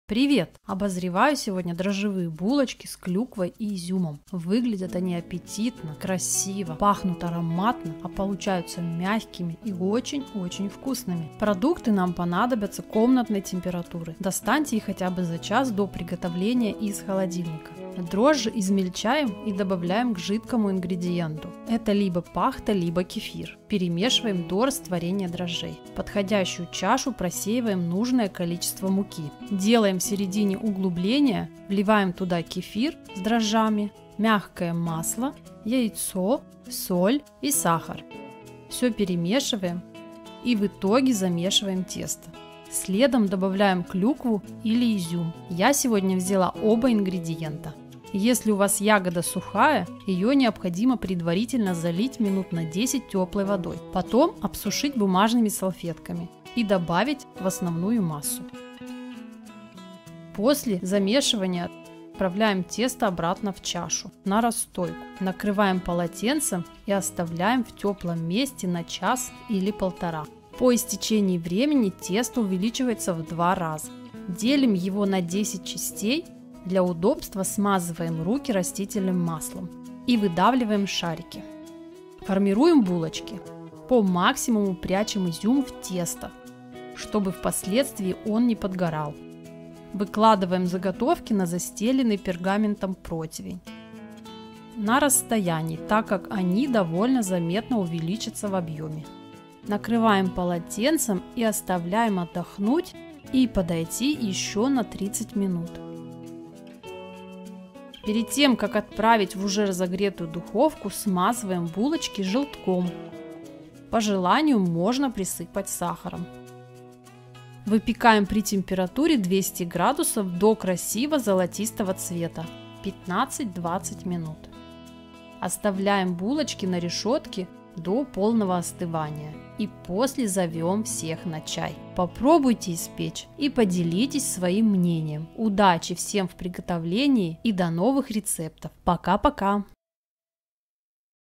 СДОБНЫЕ БУЛОЧКИ С КЛЮКВОЙ И ИЗЮМОМ /Cranberry-Brötchen aus Dinkelhefeteig
Сдобные Булочки с клюквой и изюмом, воздушные, ароматные и очень аппетитные. По-домашнему вкусно. Выпечка из полбяной муки не только вкусная, но ещё и полезная. Прекрасно подойдут  к завтраку или для перекуса.

 🍇Ингредиенты для 10 штук:

450-470 г полбяной муки (либо пшеничной)
200 мл пахты (или кефир обезжиренный)
100 г коричневого сахара (либо белого)
1 кубик свежих дрожжей (42 г)
1 яйцо
80 г мягкого масла (или маргарина)
200 г клюквы ( или изюма) либо 50/50
1 щепотка соли
1 желток (для смазывания булочек)